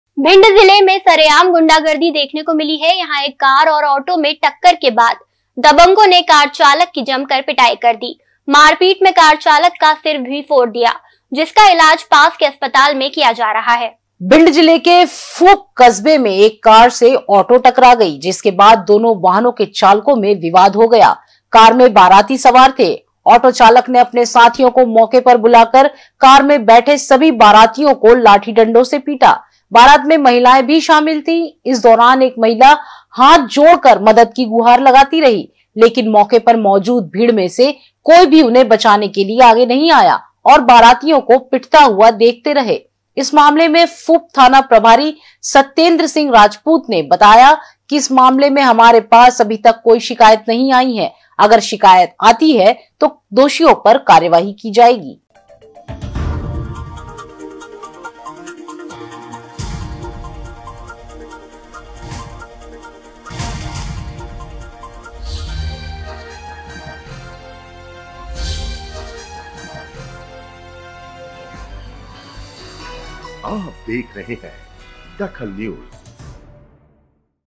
DAKHAL NEWS बदायूं से दतिया जा रहे बारातियों के साथ मारपीट
बदायूं से दतिया जा रहे बारातियों के साथ मारपीट
बारातियों को लाठी डंडों से दौड़ा-दौड़ा कर पीटा